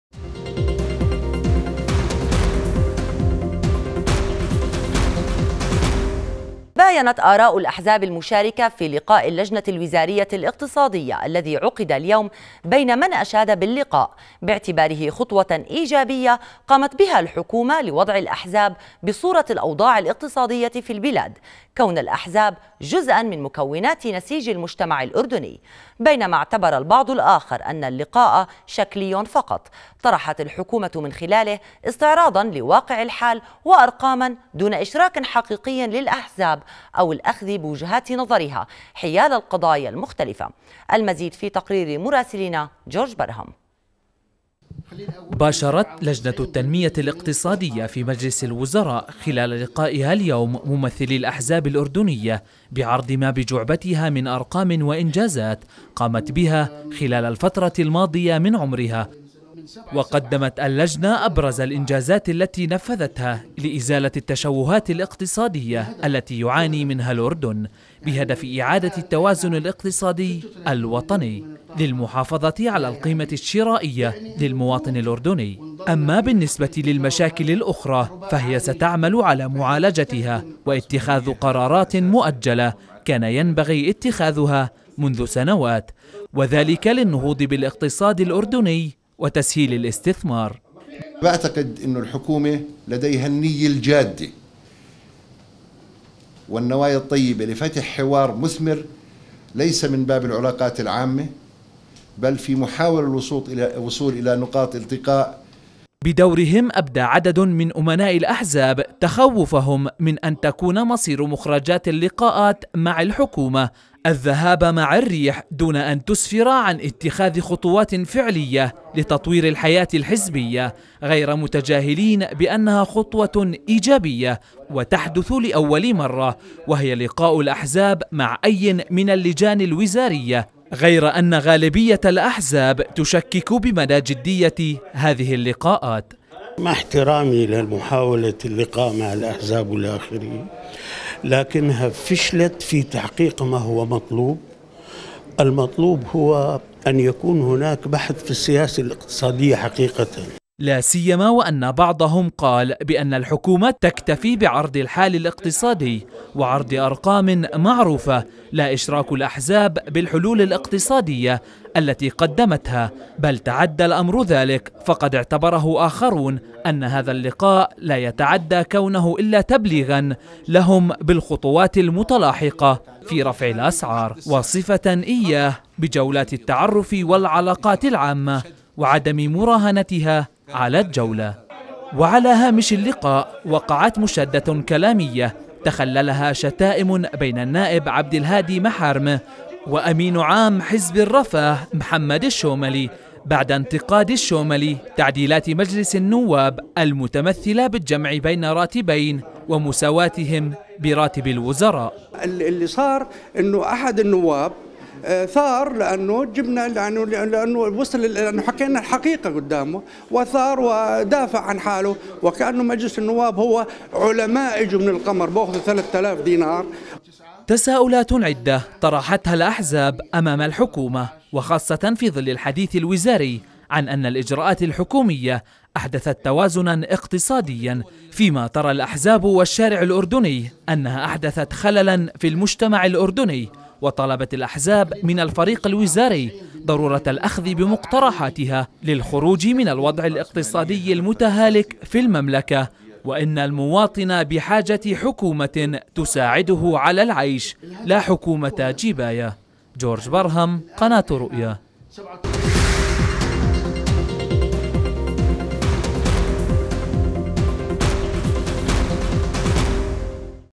الاحزاب الاردنية تصف لقاءات الحكومة بالشكلية
تباينت آراء الأحزاب المشاركة في لقاء اللجنة الوزارية الاقتصادية والأحزاب الأردنية بين من أشاد باللقاء وبين من اعتبره مجرد طرح شكلي من قبل الحكومة.

التفاصيل في تقرير الزميل جورج برهم.